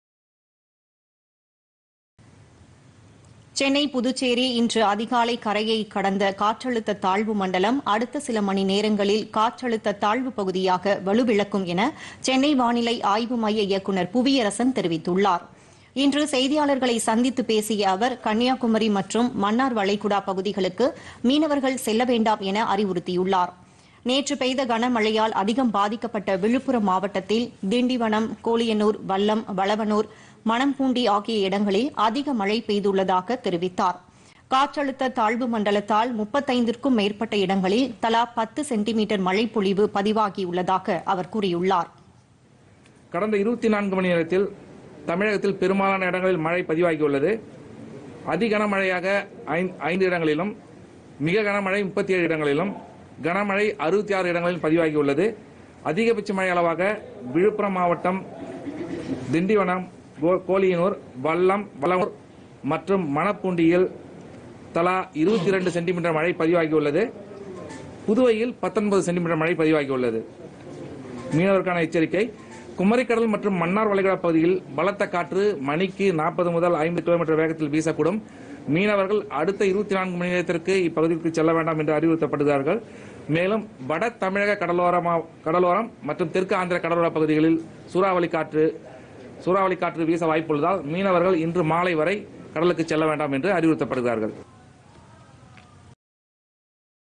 காற்றழுத்த தாழ்வு பகுதியாக வலுவிழக்கும்
சென்னை - புதுச்சேரி இடையே கரையை கடந்த காற்றழுத்த தாழ்வு மண்டலம்  அடுத்த சில மணிநேரங்களில் காற்றழுத்த தாழ்வு பகுதியாக வலுவிழக்கும் - சென்னை வானிலை ஆய்வு மைய இயக்குநர் புவியரசன்  தகவல்